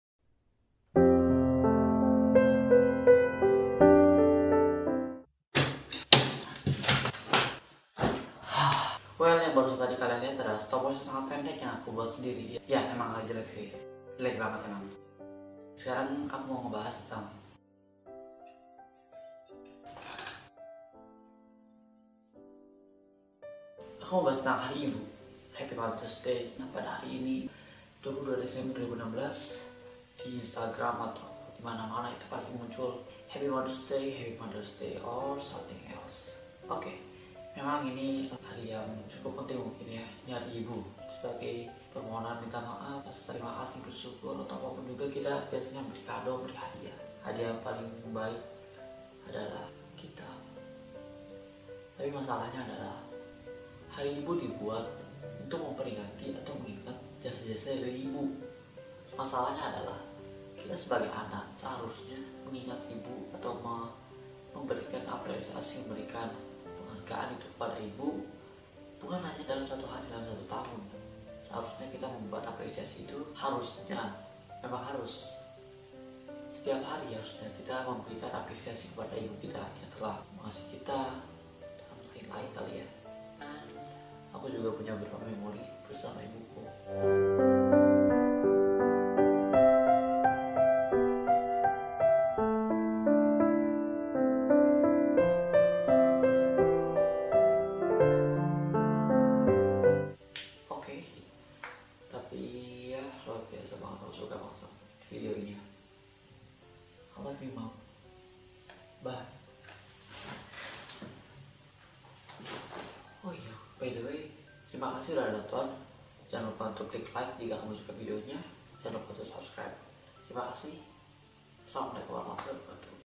Happy Mother's Day!! Selamat hari ibu - really short stop motion
Video ini  bener bener dadakan bikinnya :v ide dapet siang tadi dan langsung coba cari cari konsep. dan sebenernya konsepnya jauh dari ini tapi jadinya kaya gini, tetep enjoy videonya yaa...  BTW maaf agak telat yaa momennyaa, padahal hari ibunya udah kelewat sejam. Yang penting bisa merayakan juga he.